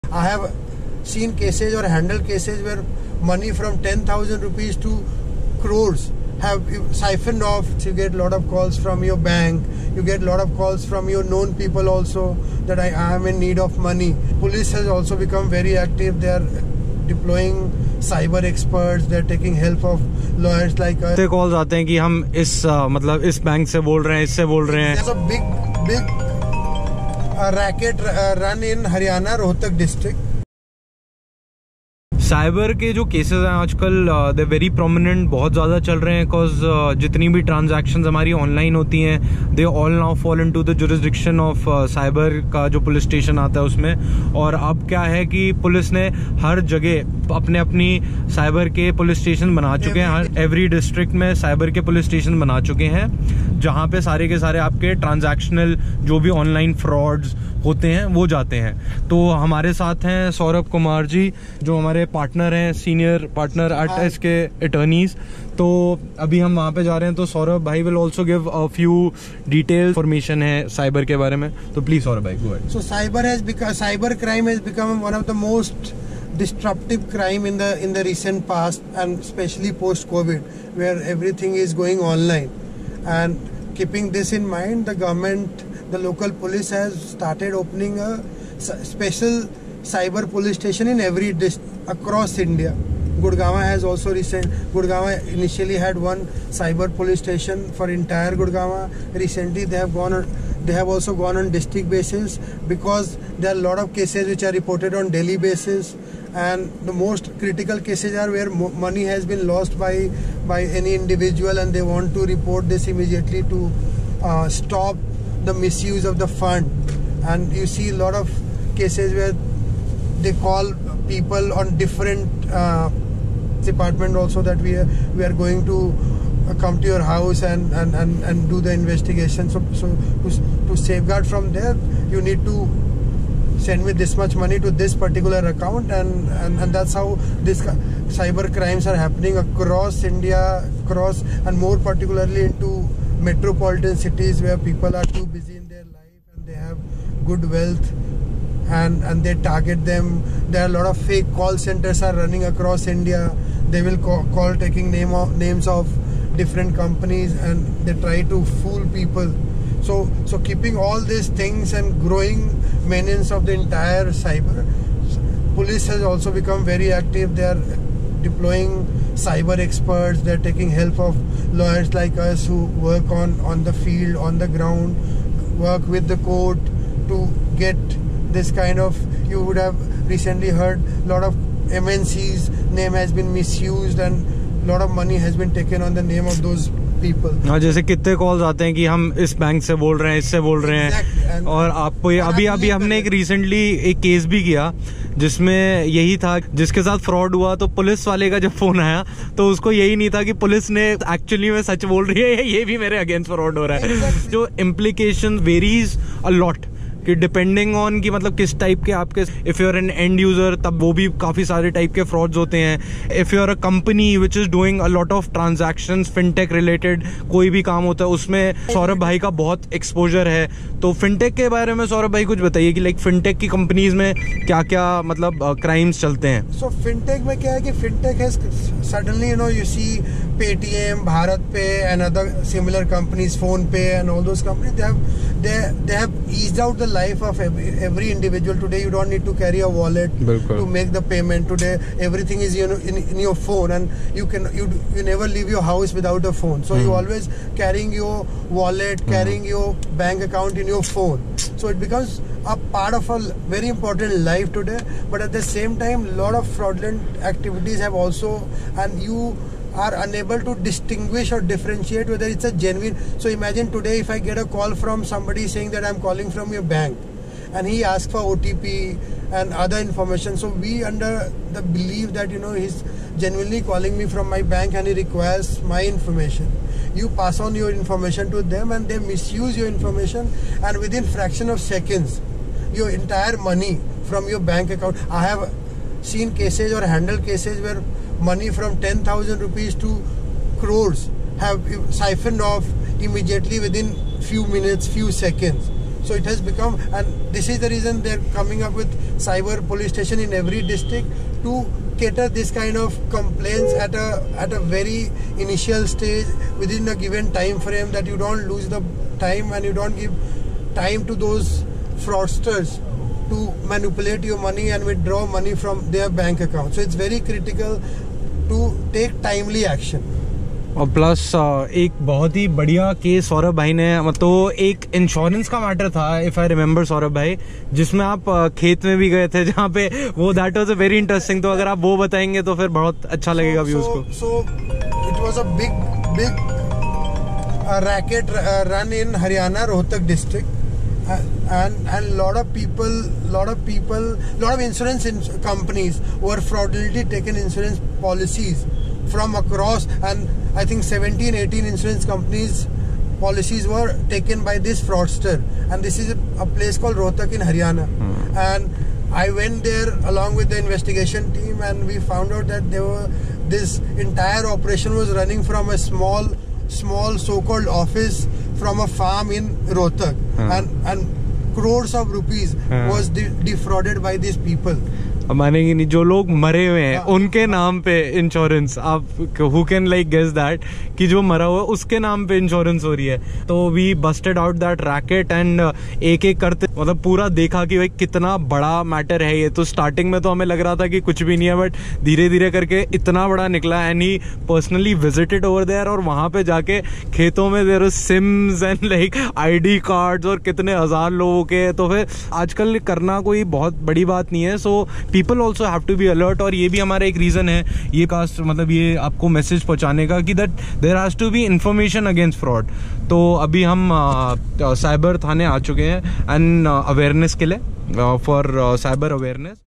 CYBER LAW AND FRUAD AND SCAMS IN INDIA|| KYA KR SAKTE HAI? | TYPES OF FRAUDS IN INDIA
Advocates Kartik Sharma and Saurabh Kumar, delve into the intricate world of cyber law, fintech frauds, and jurisdictional complexities. As we drive through the city, we engage in a riveting contestation on the types of cyber frauds, the role of police stations, and the jurisdictional challenges in cyber law enforcement.

In this vlog, we share our experiences and insights from the cases we've handled, shedding light on the real-world implications of cyber frauds. We discuss the various types of fintech frauds that are prevalent today, and how they are impacting businesses and individuals alike. We also talk about the role of police stations in handling these cases and the jurisdictional issues that often arise.

As advocates specializing in cyber law, we have a wealth of knowledge and experience to share. We believe that awareness and understanding of these issues are crucial in today's digital age. Whether you're a law student, a professional in the fintech industry, or just someone interested in understanding more about cyber law and fintech frauds, this vlog is for you.

So, buckle up and join us on this enlightening journey.